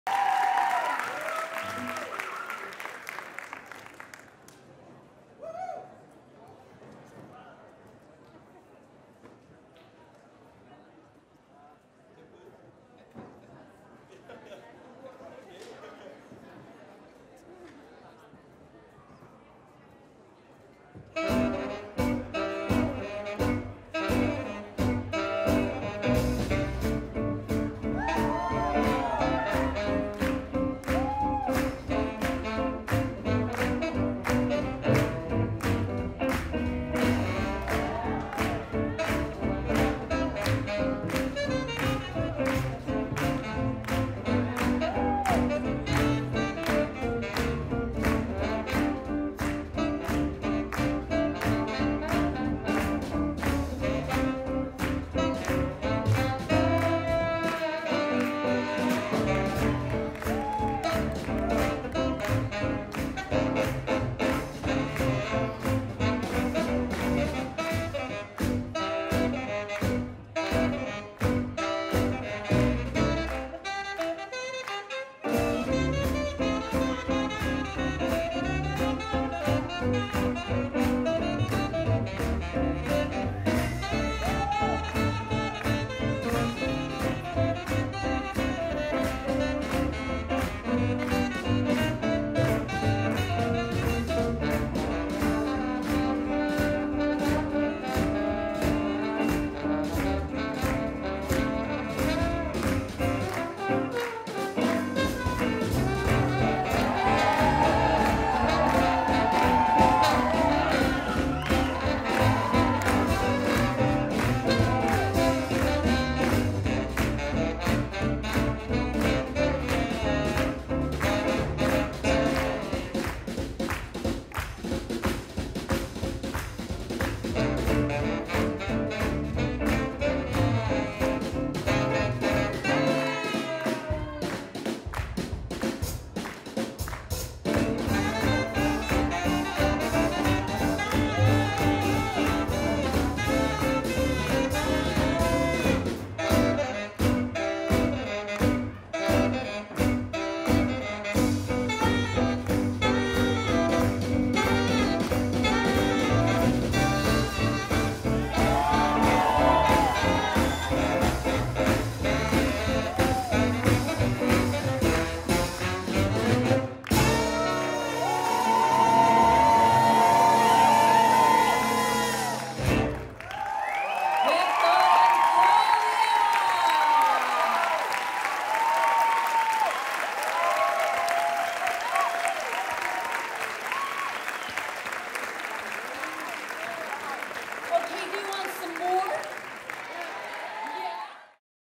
Héctor Artal & Clàudia Fonte - Swim Out Costa Brava 2019
Swim Out Costa Brava (5 - 8 September 2019), Palafrugell, Costa Brava, Spain.

Video by Within the Groove